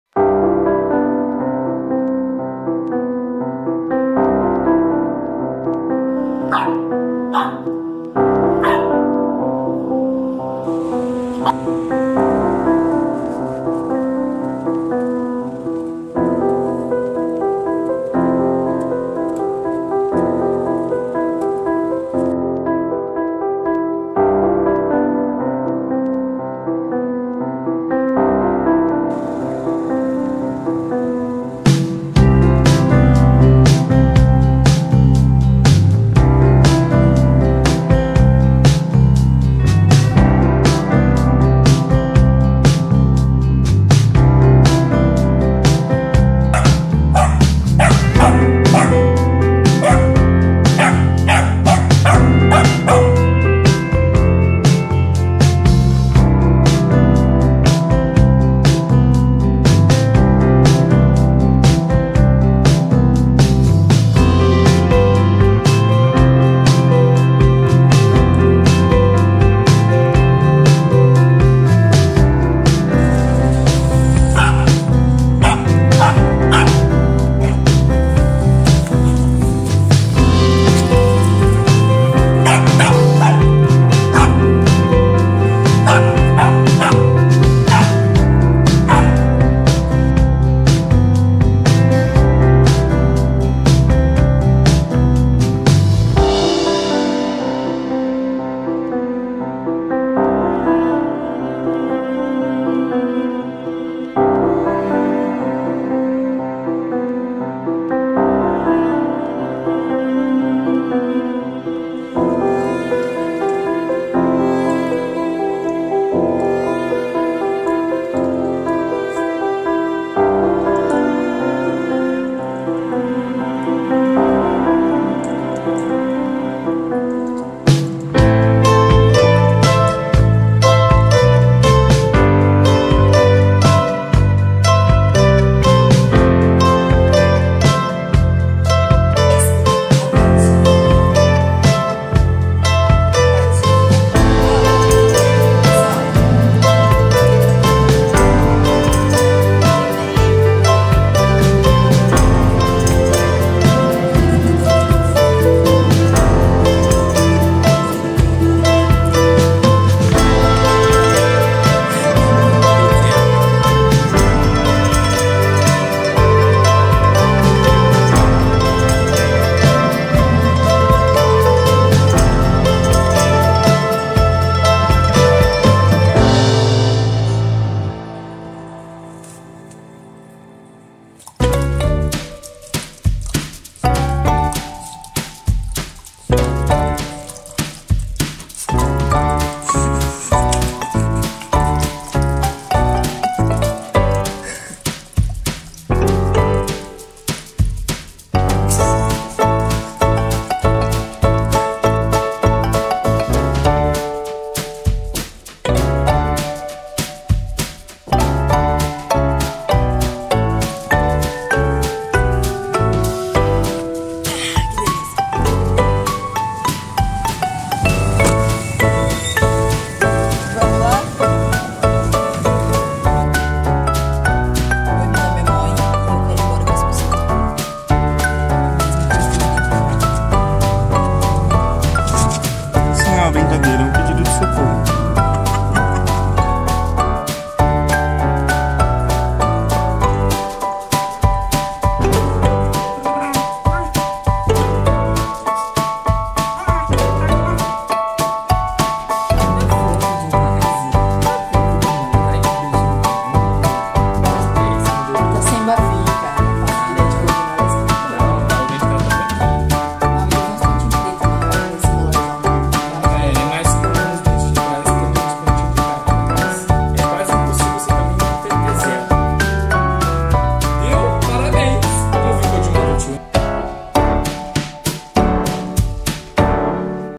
Primeiro aniversário! 🐶🥰 Moin e seu um aninho!
Hoje é um dia especial, no qual comemoramos o primeiro aninho de vida da nossa querida Moin. Trazemos aqui alguns momentos desse primeiro ano, esperamos que vocês gostem!